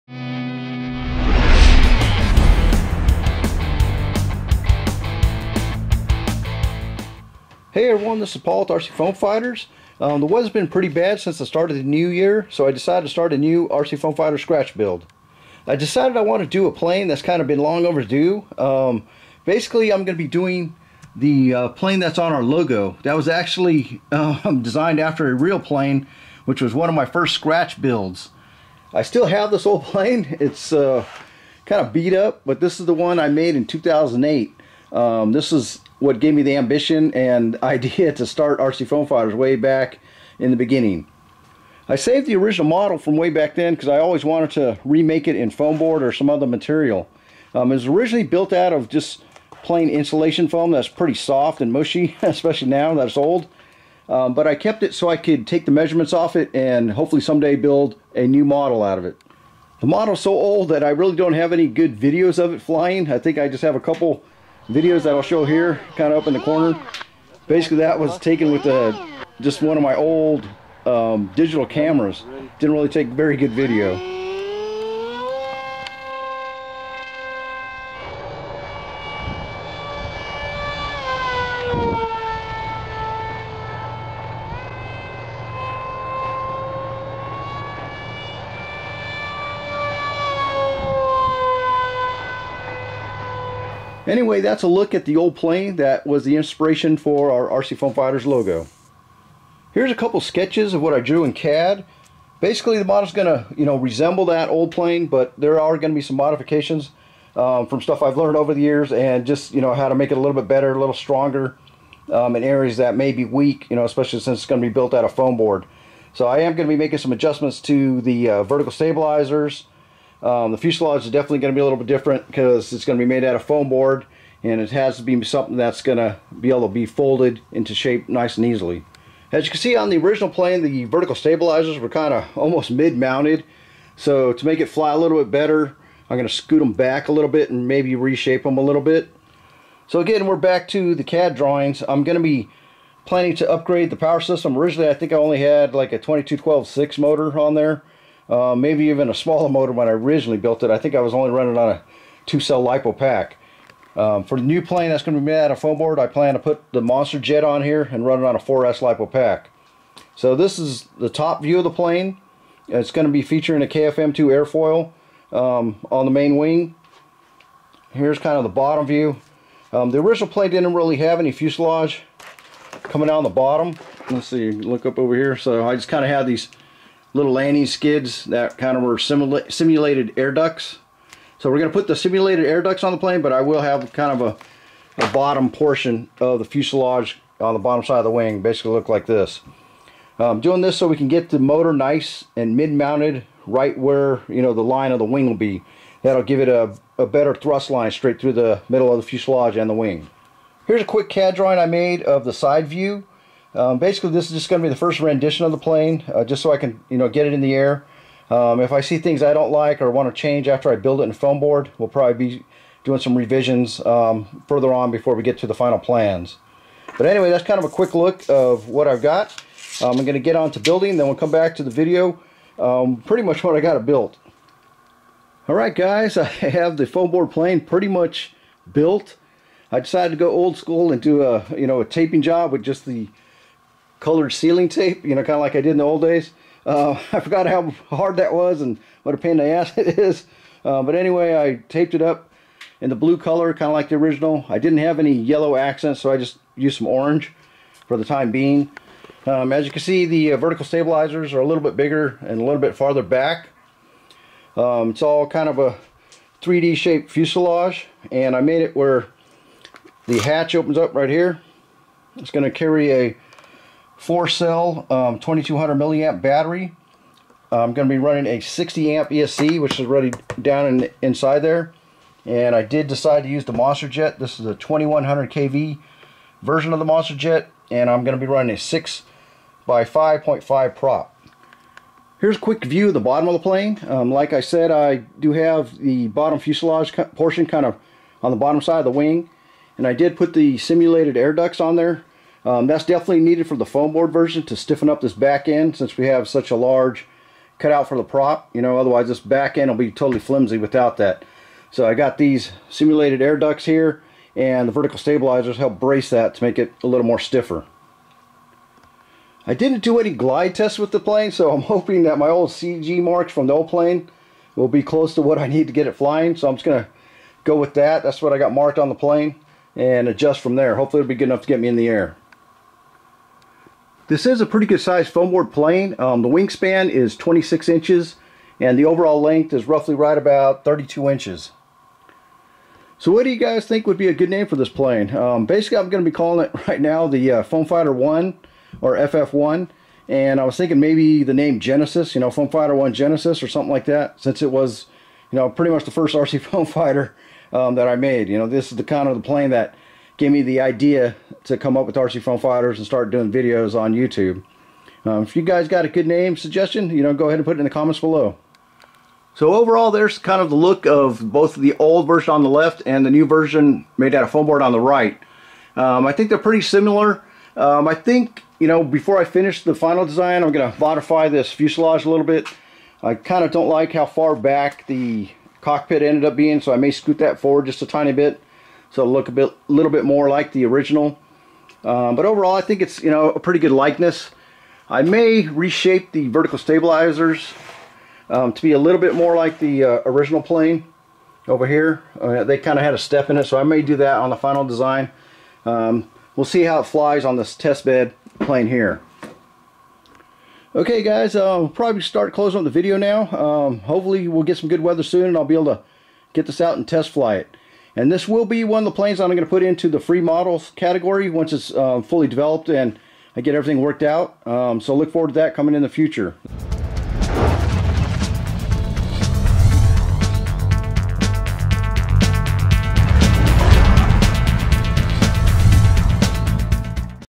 FF-1 (Foam Fighter One Test Plane)
Finally decided to try and make a foamboard version of our Logo plane. The original plane was made back in 2008 and was my inspiration to start rcFoamFighters. The test plane is complete, just waiting on decent weather to take it out for a test flight.